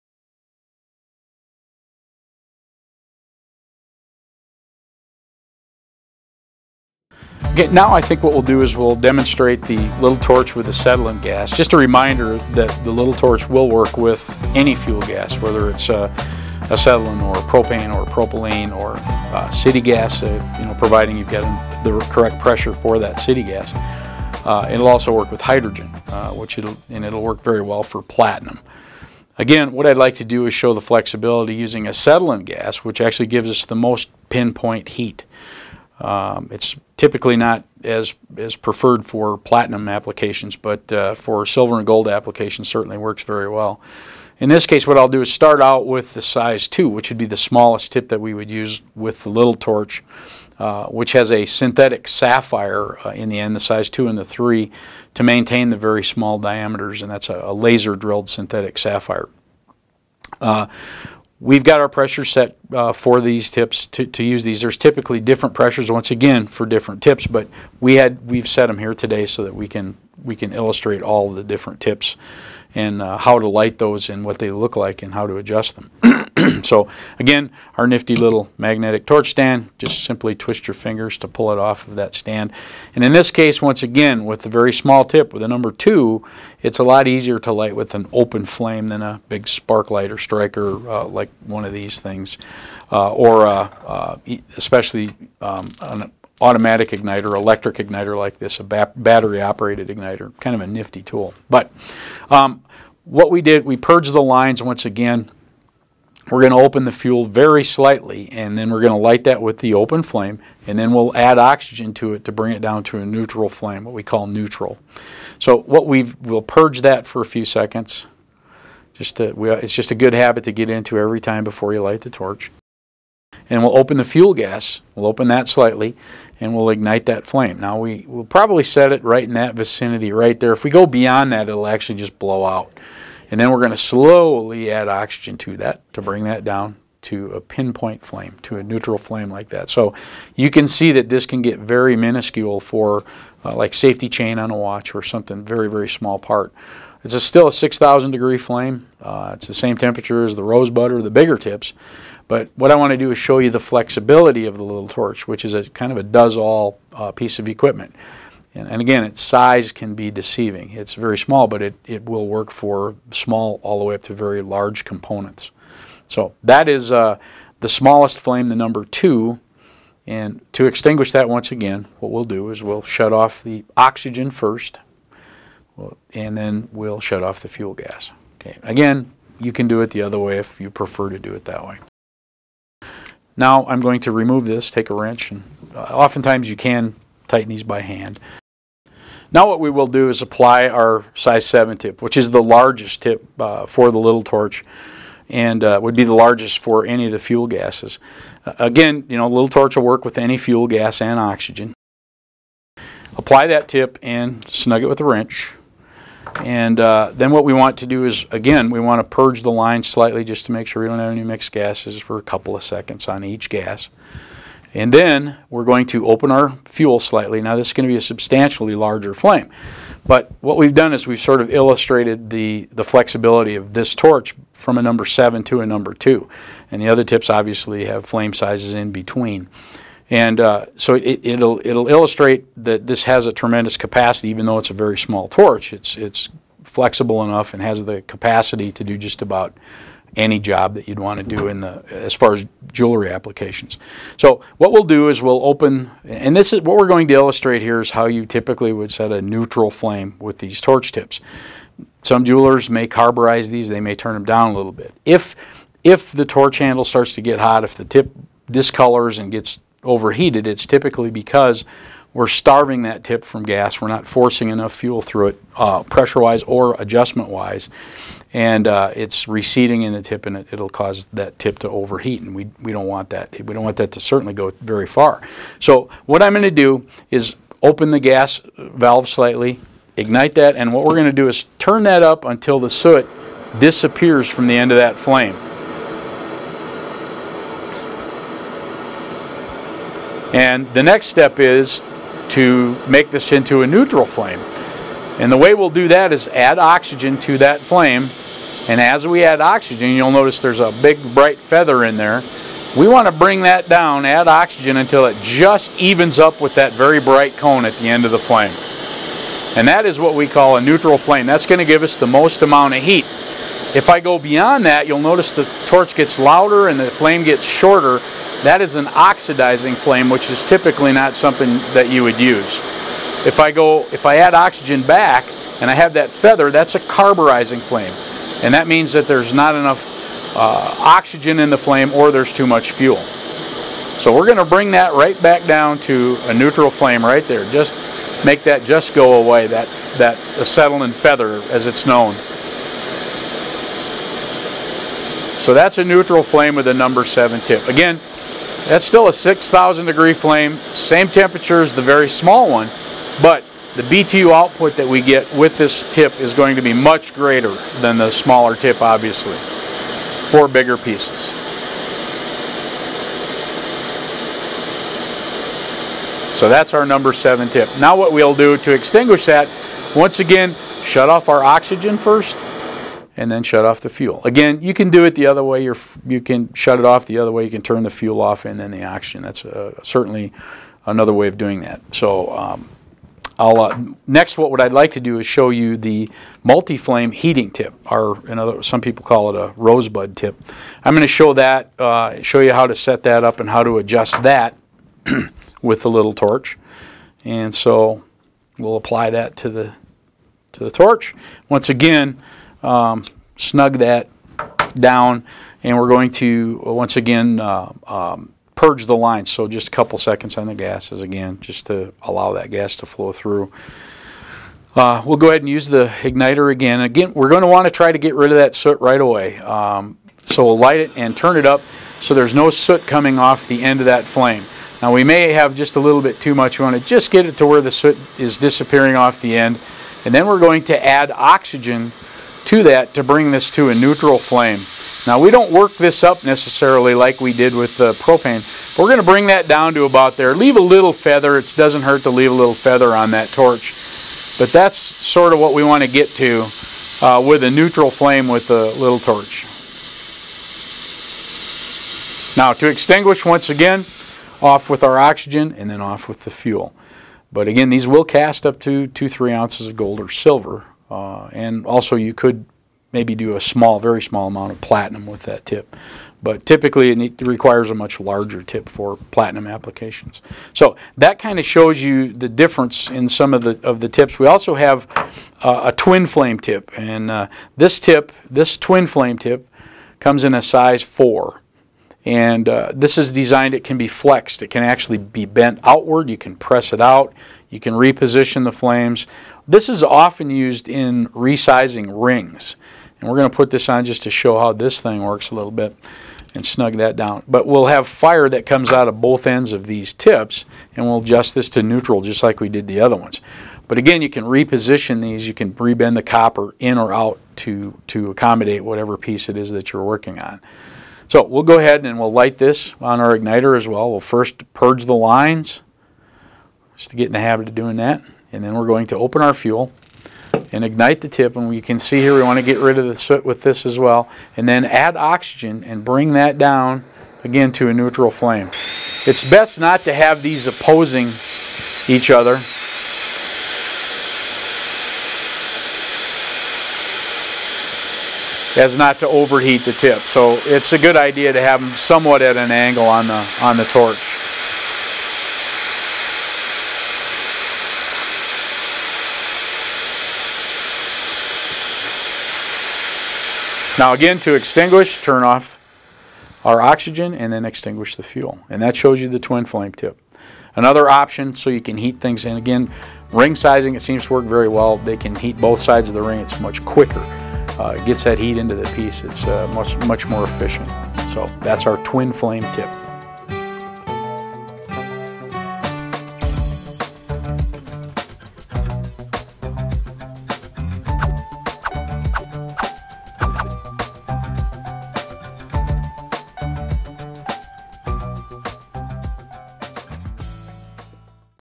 How To Light Your Smith Little Torch for Acetylene Fuel
Join Bruce Buhler of Smith Equipment as he demonstrates how to ignite and calibrate your Smith® Little Torch with acetylene gas for the best results in your jewelry-making applications. Learn about the various tip attachments available and which to use for your specific project.

Smith® Little Torch™ Acetylene and Oxygen Multiple-Orifice Melting Tip for Hydrogen or Acetylene